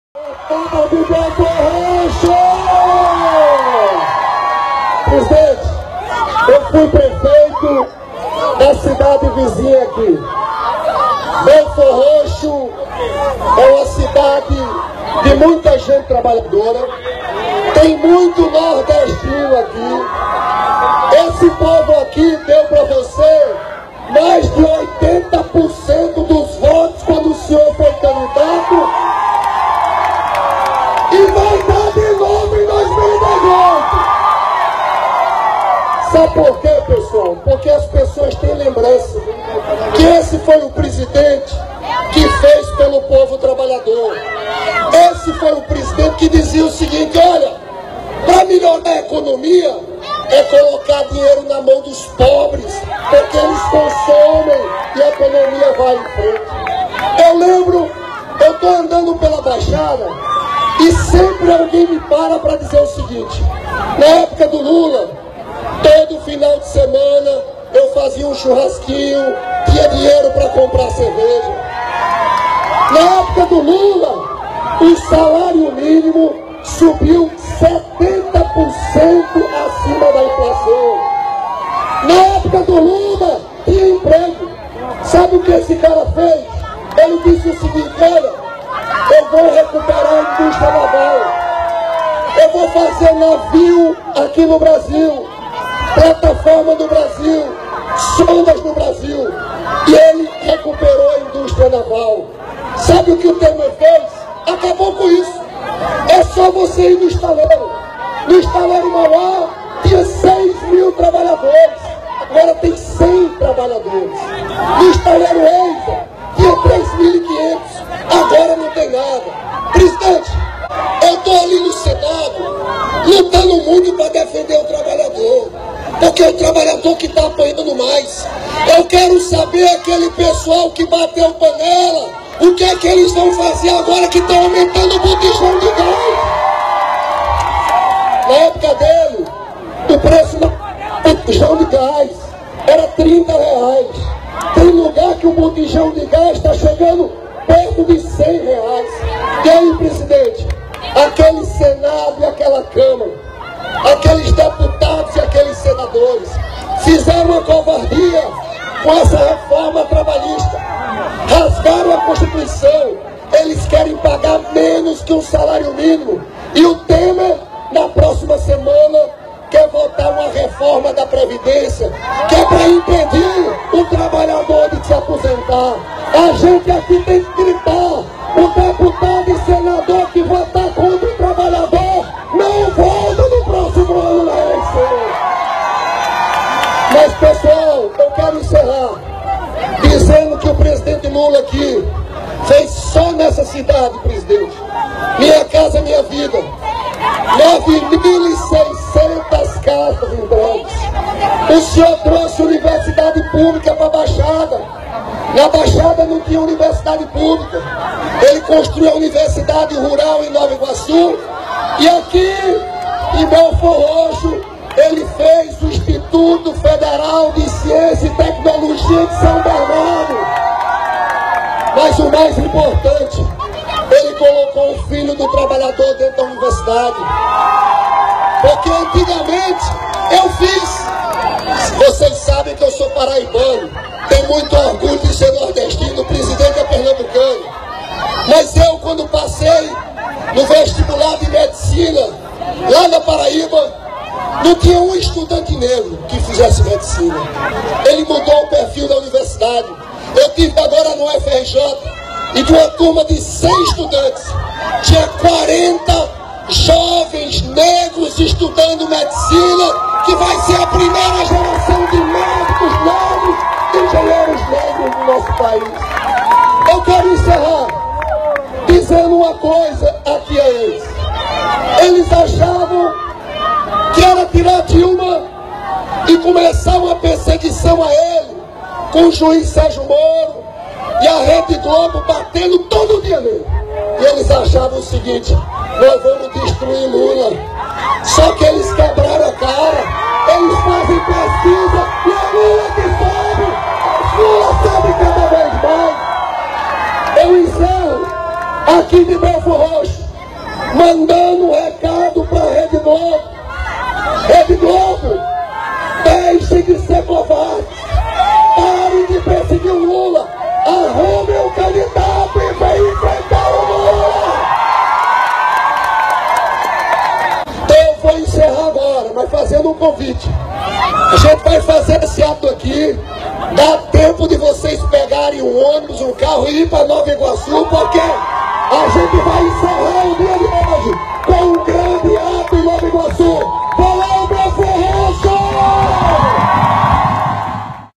caravana do Lula foi recebida pelo povo de Belford Roxo
A EMOÇÃO SÓ CRESCE! A caravana do Lula foi recebida com muita esperança pelo povo de Belford Roxo porque a Baixada Fluminense quer voltar a sorrir e ser feliz. Já estamos a caminho de Nova Iguaçu. Aguenta coração!